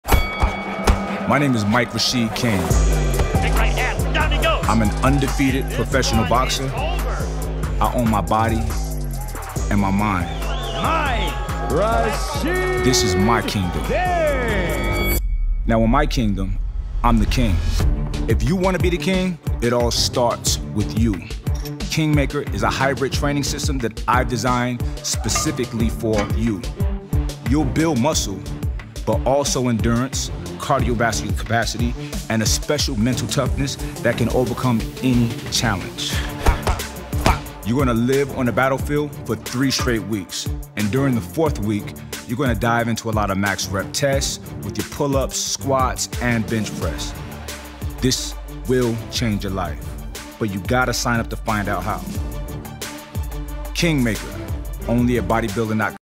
Kingmaker: 4 Weeks to Fighting Shape | Trailer
Rule the gym. Rule your life. Fitness legend Mike Rashid created the perfect plan to be ready for anything–and look the part.

Everything he’s learned from competitive bodybuilding, powerlifting, boxing, and just hanging out in the most badass gyms in the world comes together in this incredible four-week gauntlet.

| 4 Weeks of Workouts |
You’ll mix classic barbell work, complexes, high-rep bodyweight finishers, and old-school conditioning to burn fat and build your lungs, legs, and will. Do Kingmaker for four weeks, and you’ll love how you look and feel. Turn around and repeat it for another four, as Mike recommends, and you’ll be a warrior through and through.

| Expert Instruction |
ike Rashid isn’t into just "show" or "go." This is his master plan for having it all! Every movement features a demonstration, and he'll guide you through every set and rep of the first workout with a complete workout video.